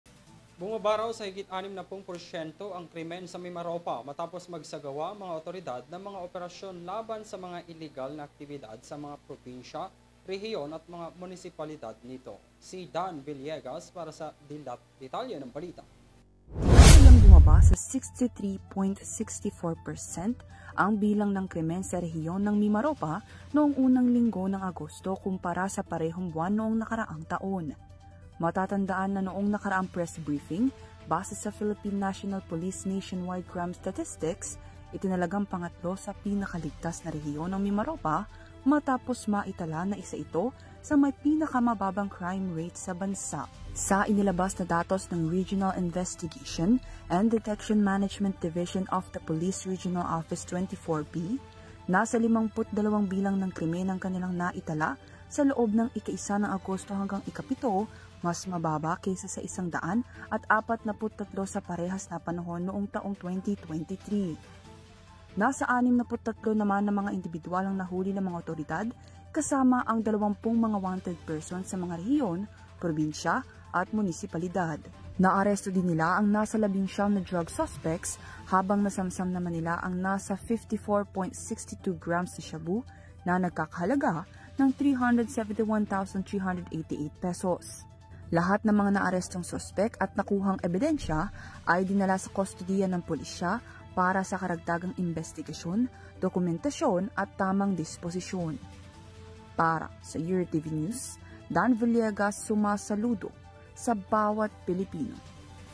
KRIMEN SA MIMAROPA, BUMABA NG MAHIGIT 60% -- PDEA
BUMABA SA HIGIT ANIM NA PUNG PORSYENTO ANG KRIOMEN SA MIMAROPA MATAPOS MAGSAGAWA ANG MGA AWTORIDAD NG MGA OPERASYON LABAN SA MGA ILEGAL NA AKTIBIDAD SA MGA PROBINSYA, REHIYON, AT MGA MUNISIPALIDAD NITO.